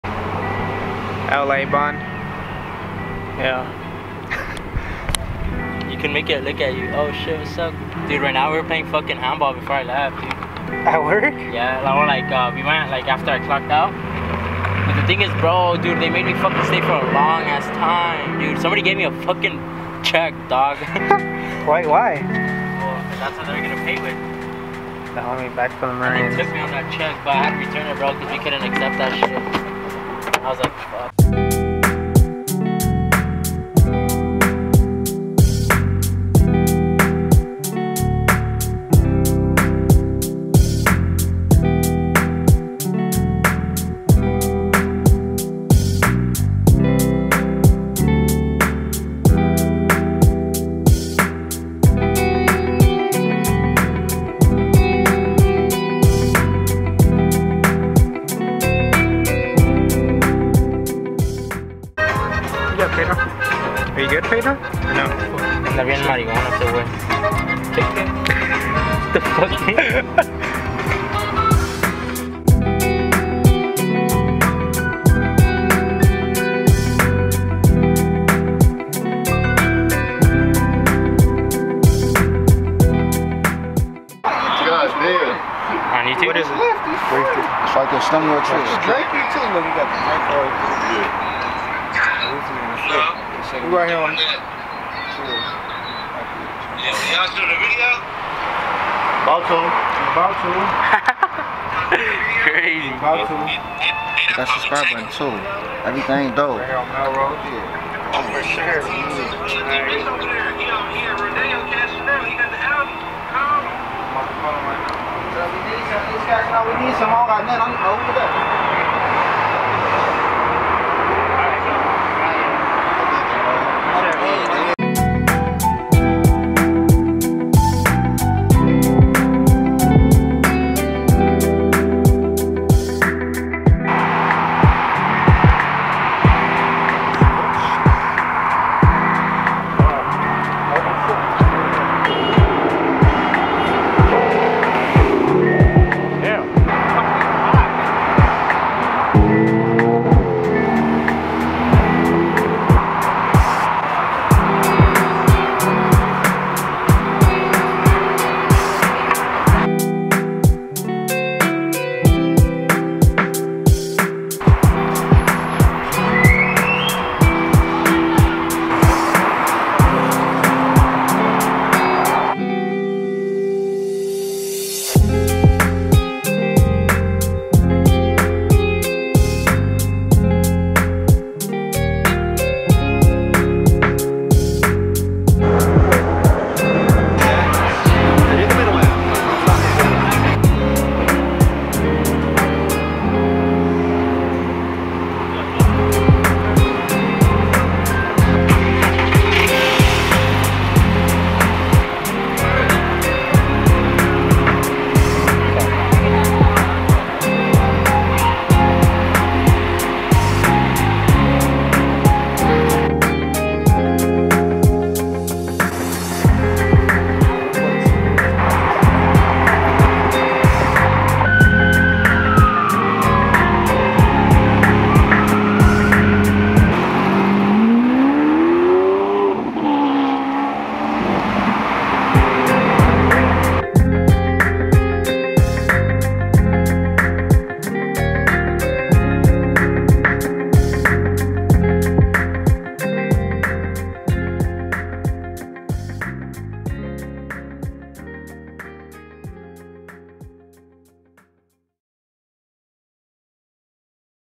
Fairfax & Melrose at 8pm |LA BOUND
Went to LA. Melrose & Fairfax after 8pm.